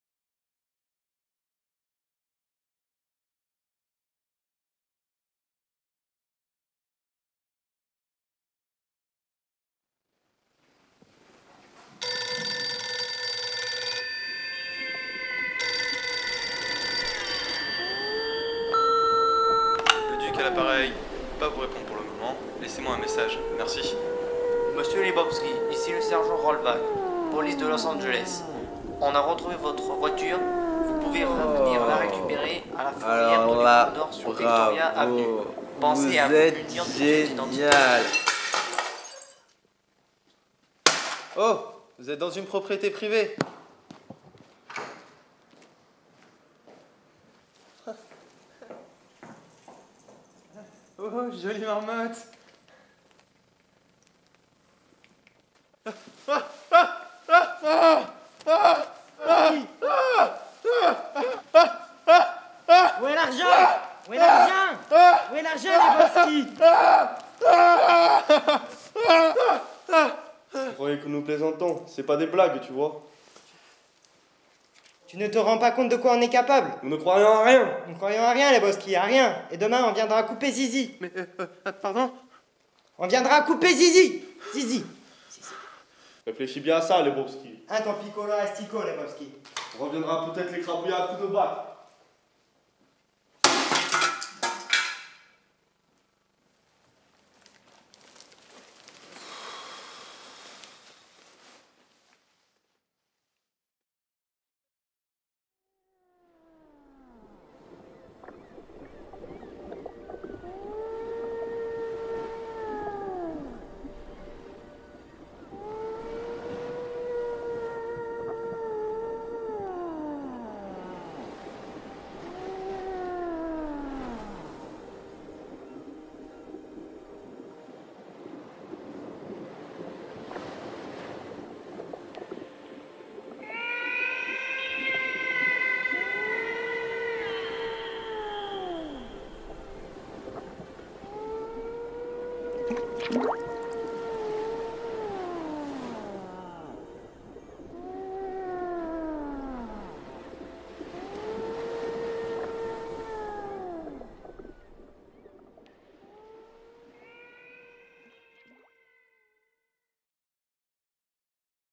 Atelier film suédé - Lycée Périgueux (2016)
Film suédé réalisé avec les lycéens de La Réole,
d'après The Big Lebowski (Frères Cohen)
En partenariat avec ALCA.